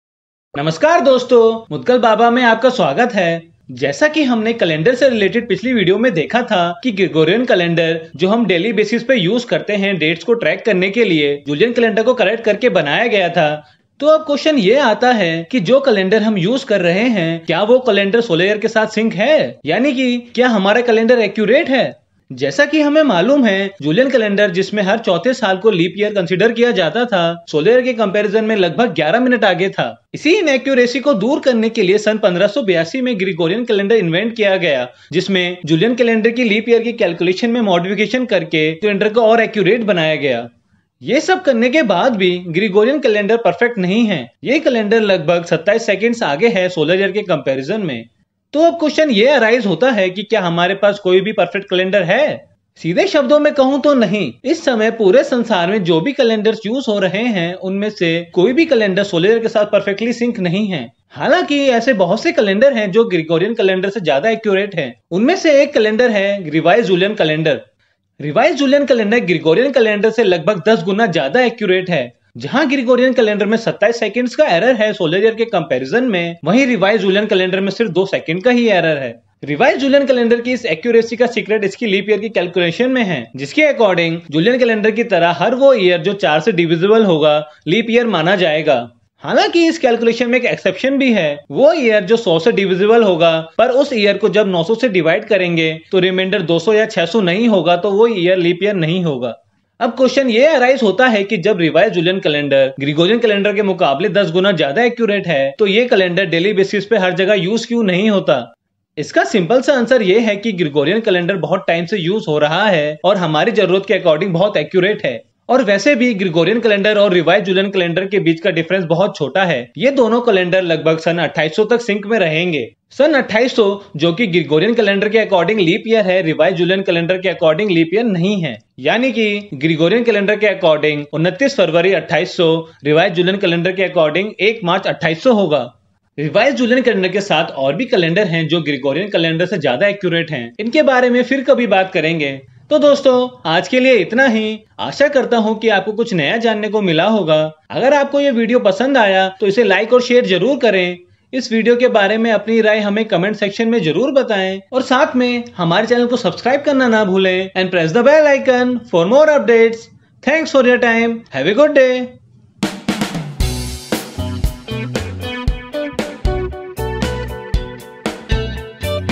Kya hamara Calendar accurate hai? | History of Calendars - part 3 |Mudgal Baba
In our last part in the series "History of Calendars", we have discussed how we get our Modern calendar. In this video, we will talk about the accuracy of our Modern Calendar whether this calendar is accurate or not.
Watch the video to know, hope you will get to know something new.